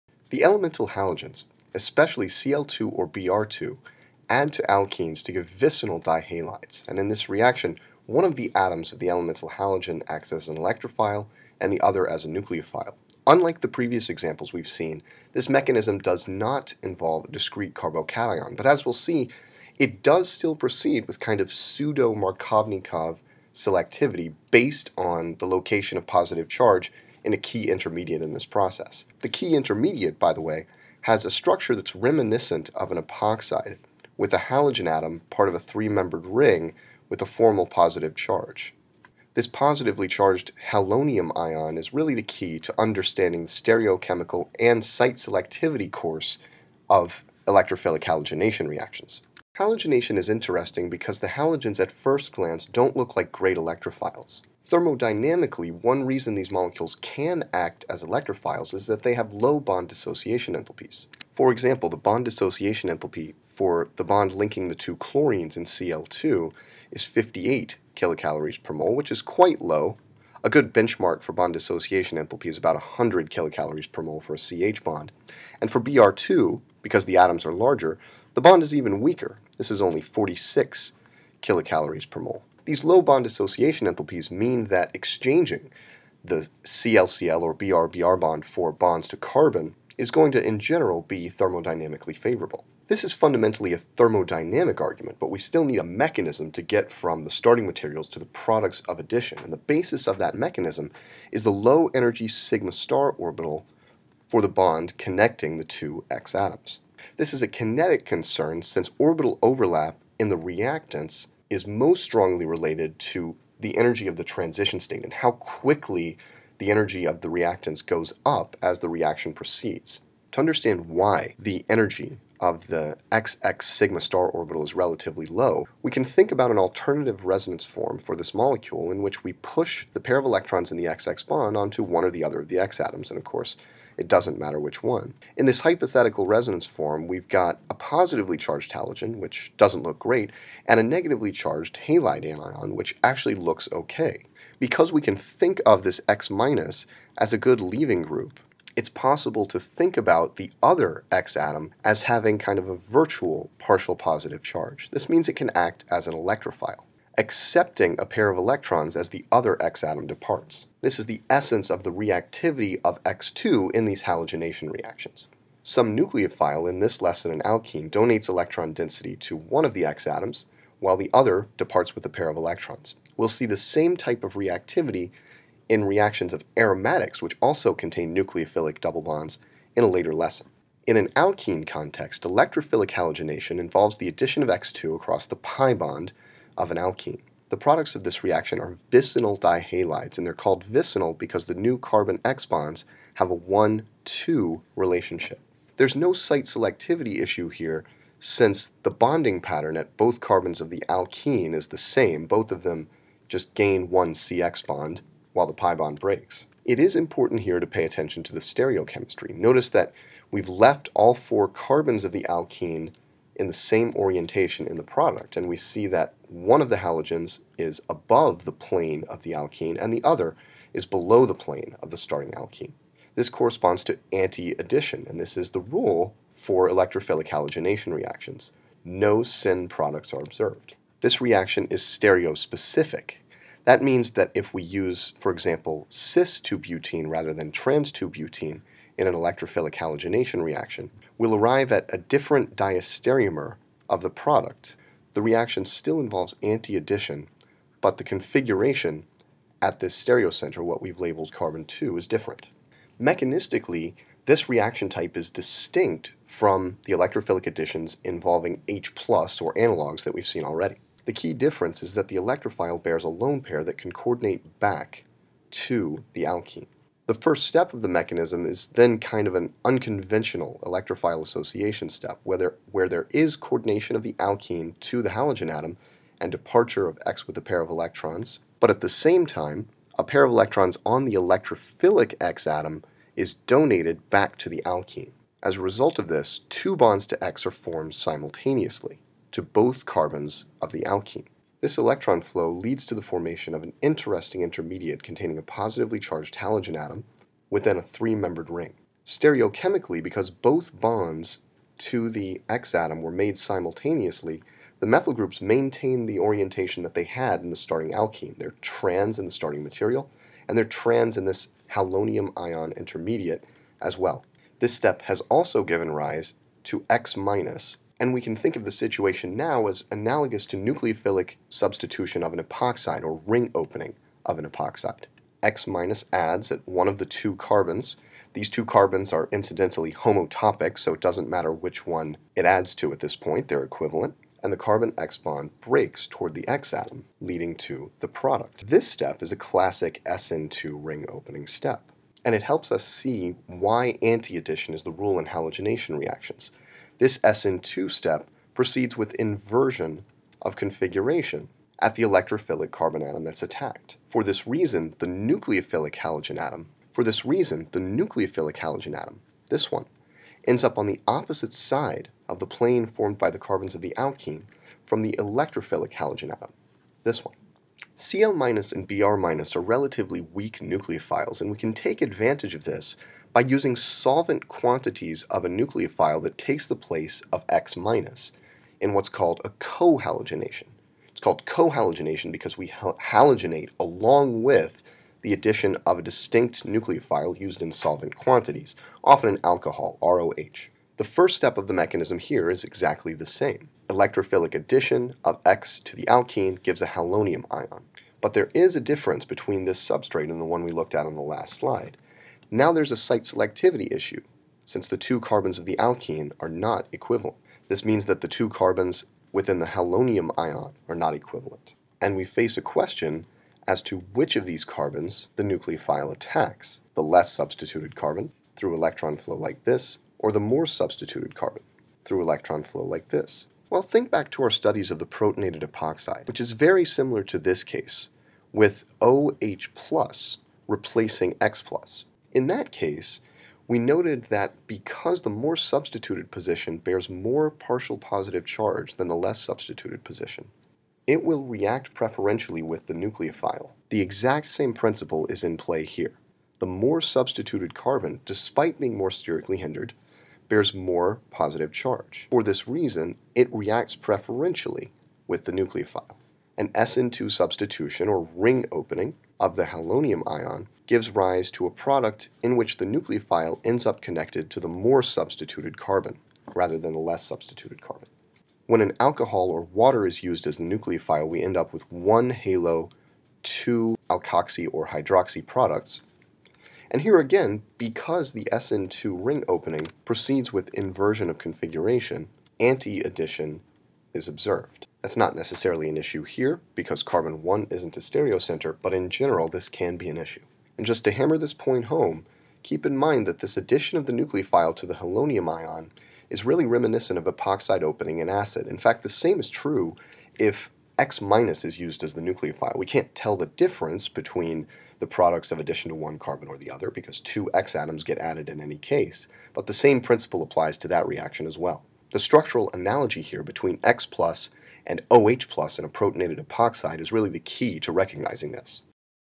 23.04 Additions of Elemental Halogens
Halogenation and cohalogenation. Products and mechanism. Positive charge distribution and site selectivity in openings of halonium ions.

00:00 Introduction
00:49 Elemental Halogens as Electrophiles
03:00 Addition of Halogens to Alkenes
04:12 Reaction Mechanism
06:07 Cohalogenation